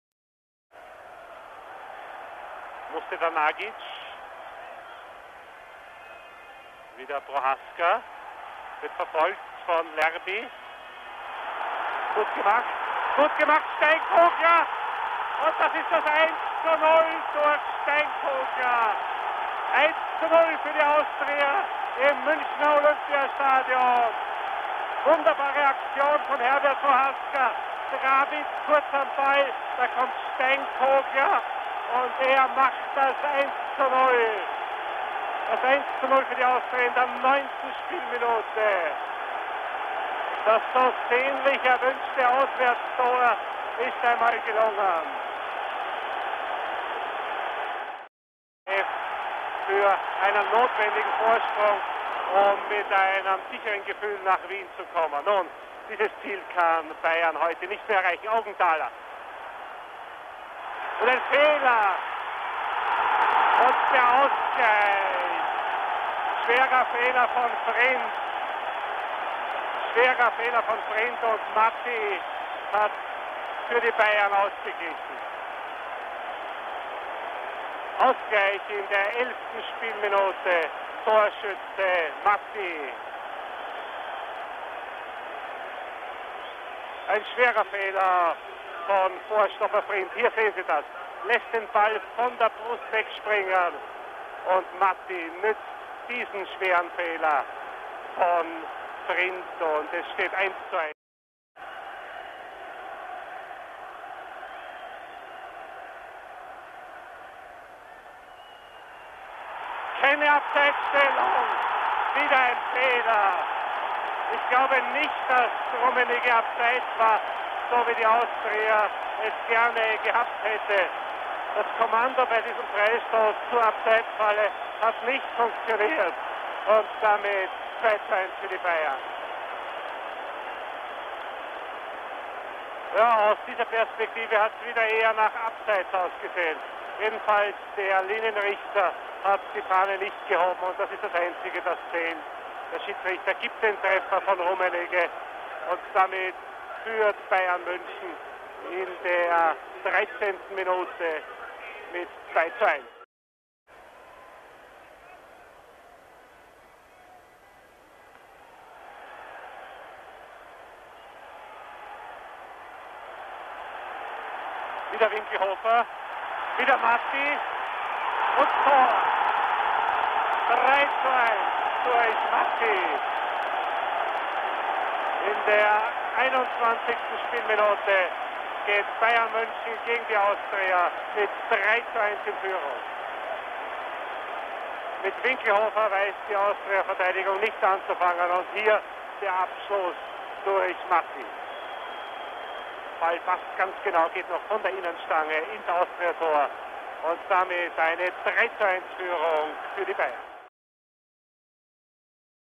Bayern v Austria 1985 (Pt. 1)
Bayern Munich v Austria Memphis Vienna
23 October 1985, European Champions' Cup,
2nd round, 1st leg, attendance: 64,000
Referee: Daina (Switzerland)

Bayern:
1 Pfaff - 2 Winklhofer, 5 Augenthaler, 4 Eder, 3 Pflüg-
ler - 7 Beierlorzer, 10 M.Rummenigge, 6 Lerby,
11 Nachtweih - 9 Mathy, 8 F.Hartmann (58. Kögl)

Austria Memphis:
1 Resch - 2 Dihanich, 3 Obermayer, 5 Frind, 6 Tür-
mer - 11 Mustedanagic, 8 Prohaska, 10 Nyilasi -
9 Polster, 7 Drabits, 4 Steinkogler